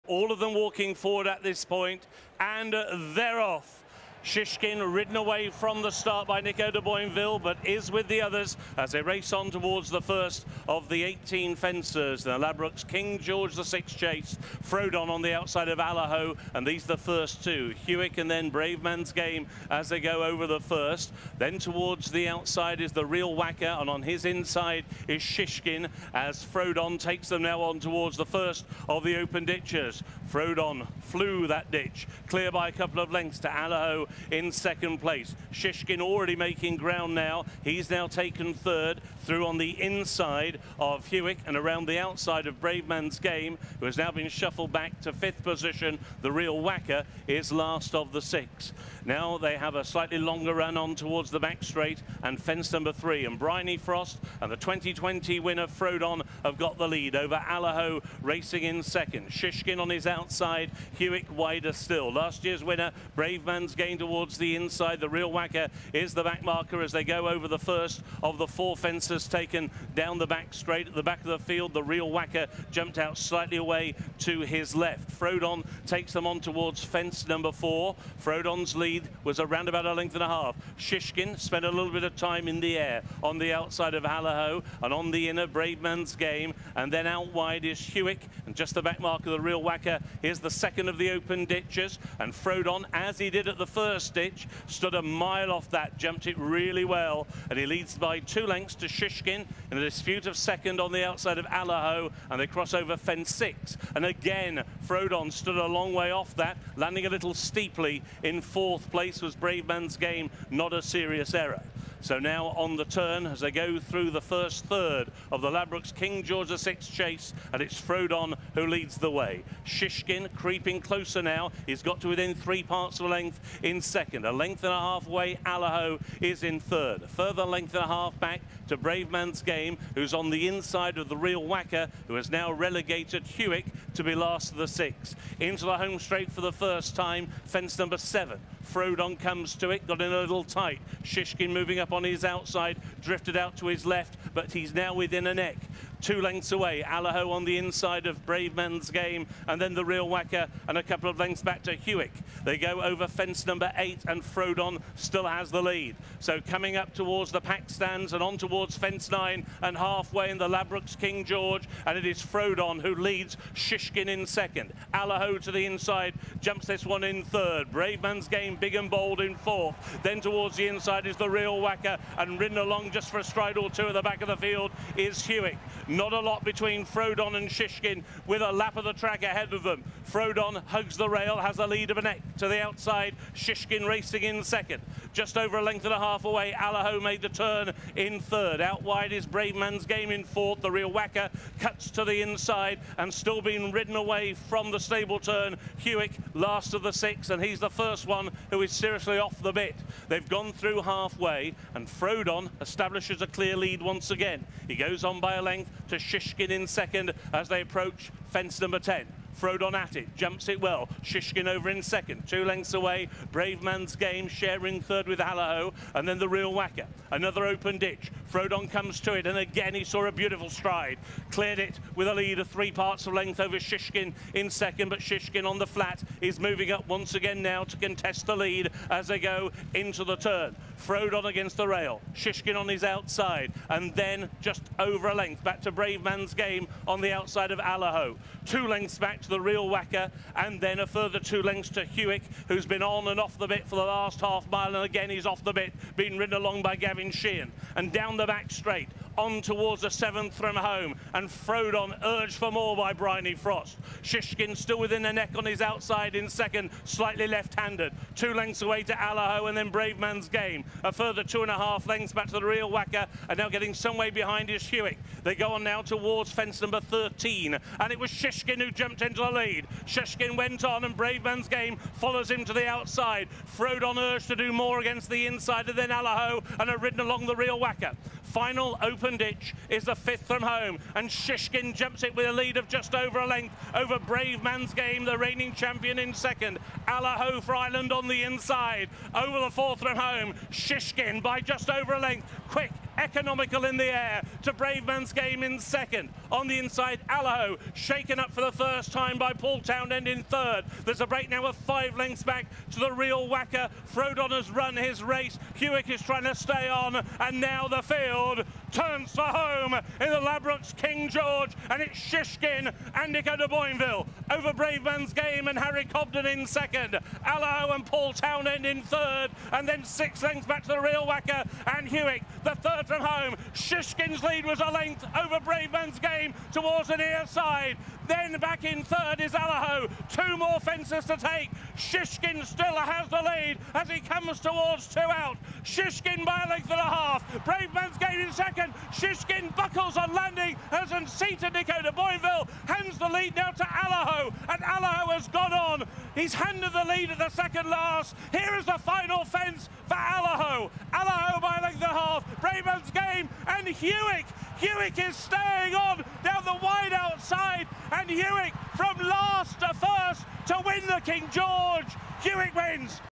ABSOLUTE DRAMA IN THE LADBROKES KING GEORGE! HEWICK STRIKES LATE
Hewick stayed on sensationally to win the Ladbrokes King George VI Chase at Kempton Park as Shishkin unseated his jockey at the second-last.